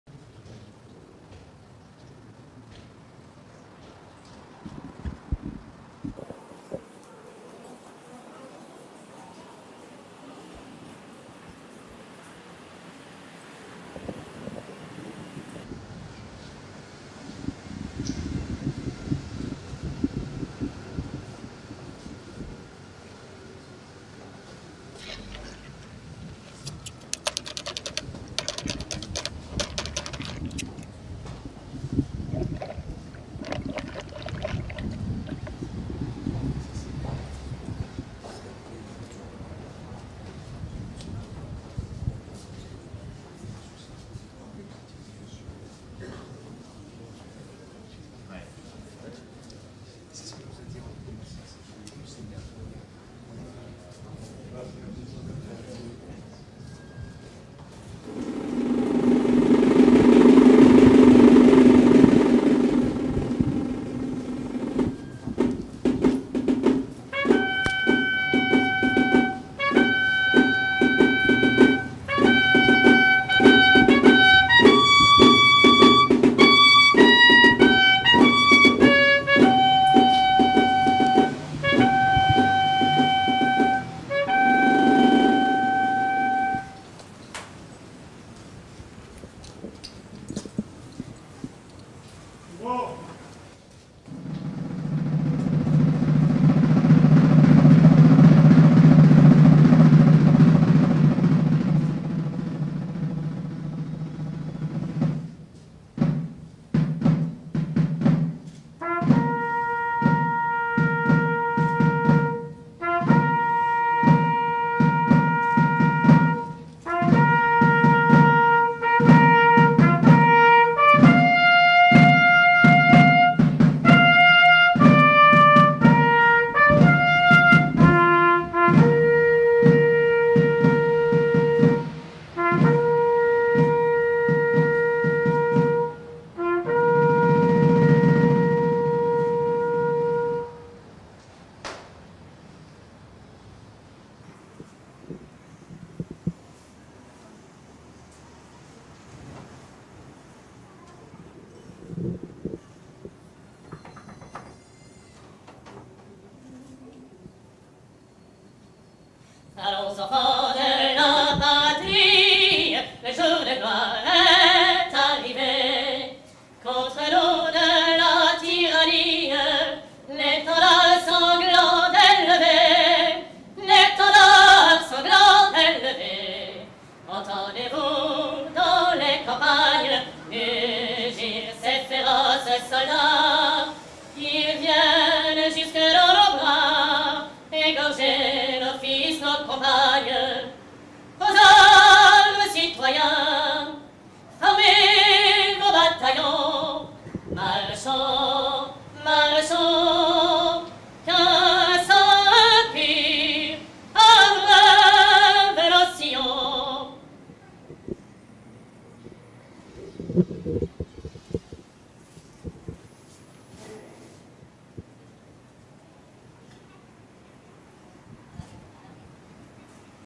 Macron, Sarkozy et Darmanin à la Grande Mosquée de Paris
Pour l'anniversaire des 100 ans de la Grande Mosquée 🙏 de Paris.
Emmanuel Macron, Nicolas Sarkozy et Gérald Darmanin sont réunis.

Ps: Avec une petite touche d'Humour en ces temps difficiles.